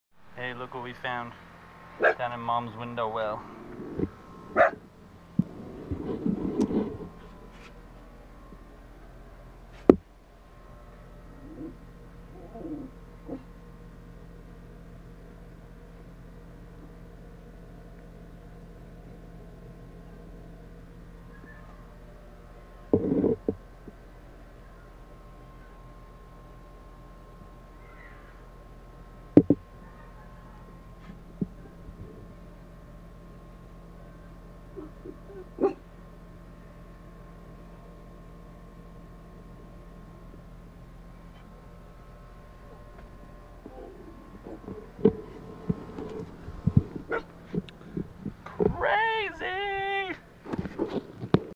Rare! Albino Skunk Fell in Window Well...Now What?
We found an albino skunk in the window well during our 4th of July party.  It climbed out a short time later after we fed it and gave it water.